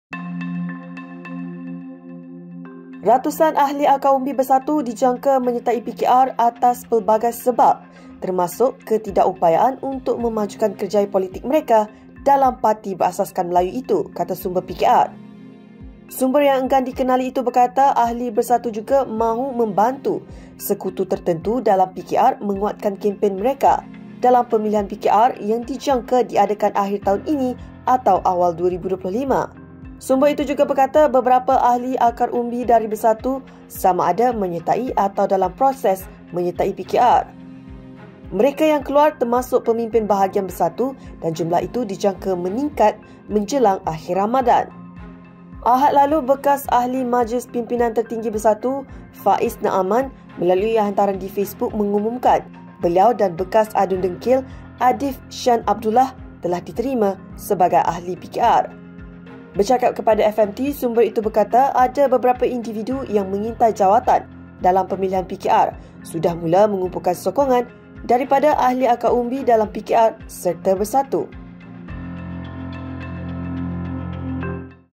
Ratusan akar umbi Bersatu dijangka sertai PKR, kata sumber
Ratusan ahli akar umbi Bersatu dijangka menyertai PKR atas pelbagai sebab, termasuk ketidakupayaan untuk memajukan kerjaya politik mereka dalam parti berasaskan Melayu itu, kata sumber PKR.